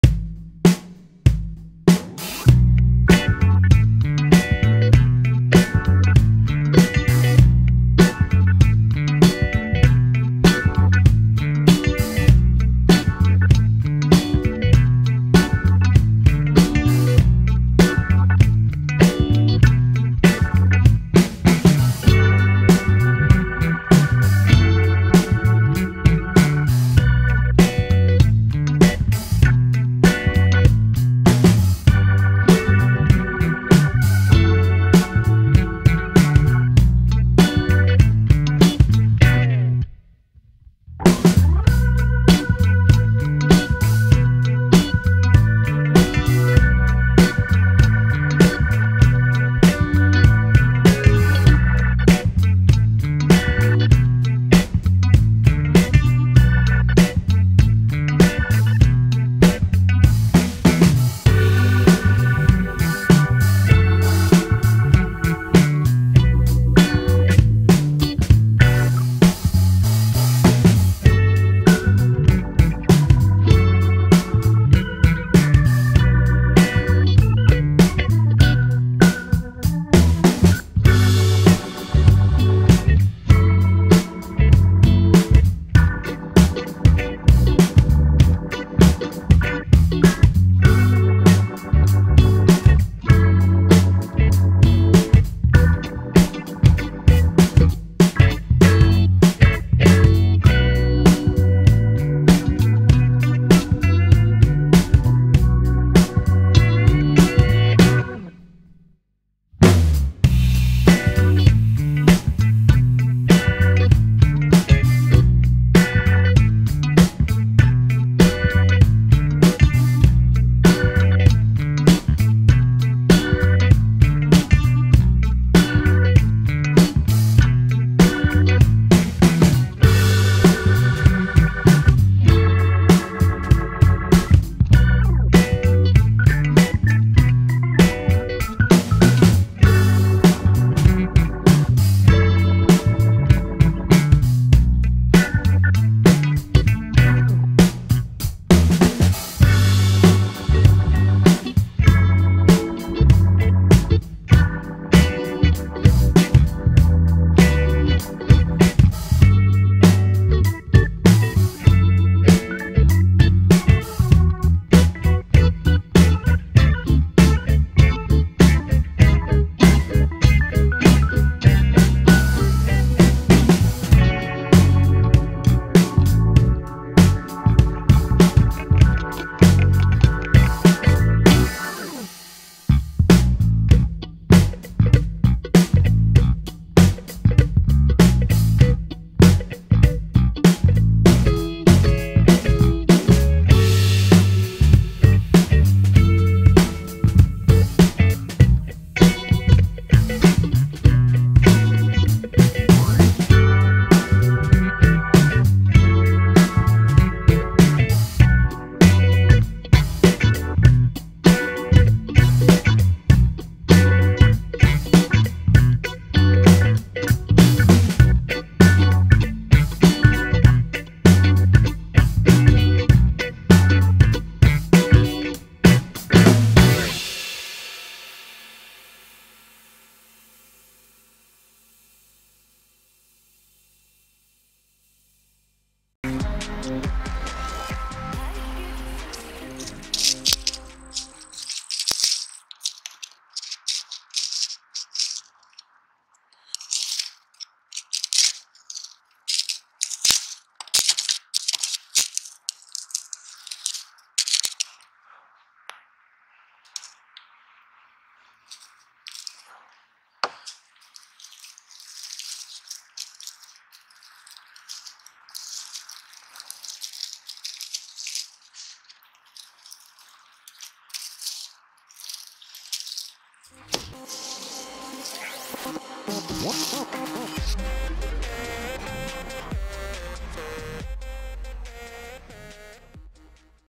matchesdomino 20000 Match Chain Reaction, matchstickcannоn
Match Chain Reaction VOLCANO ERUPTION Amazing Fire Domino

About 20000 matches were used during this video. The creation of this volcano took a lot of time and effort.

matches domino

matches domino
matchesdomino